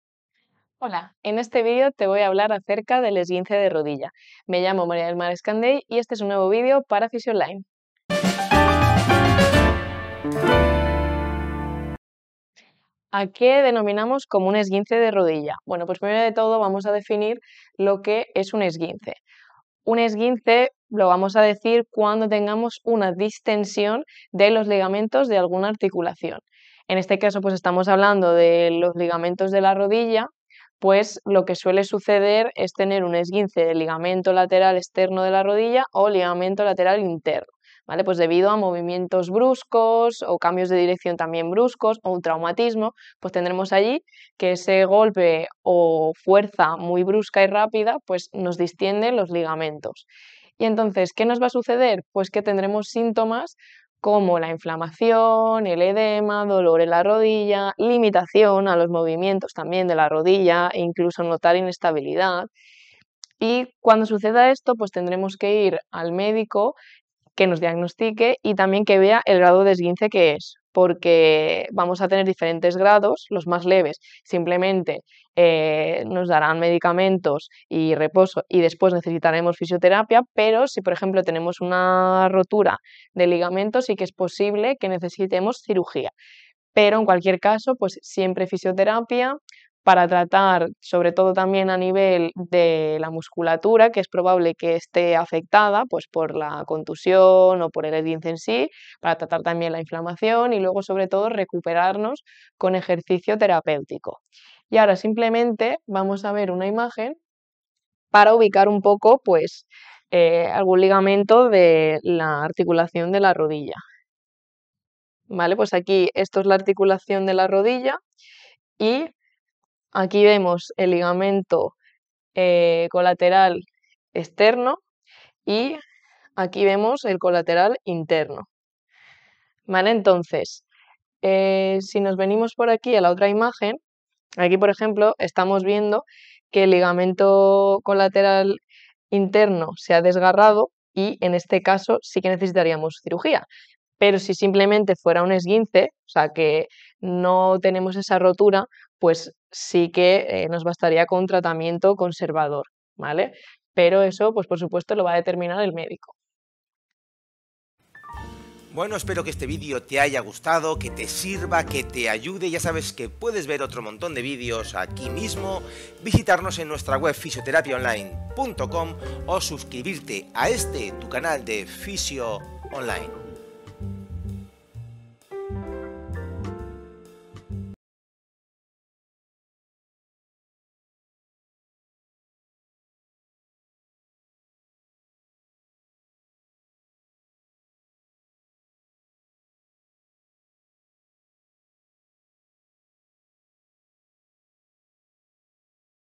ESGUINCE DE RODILLA | ¿Qué es?
↪️ Se trata de una lesión bastante común que se presenta dentro de la población jóven y adulta de edad media sobre todo que practiquen algún tipo de deporte que involucre la carrera y los cambios de dirección en donde se evidencia una distensión parcial o total de los ligamentos que estabilizan la rodilla de manera pasiva

00:00 Intro
00:14 Definición
02:38 utro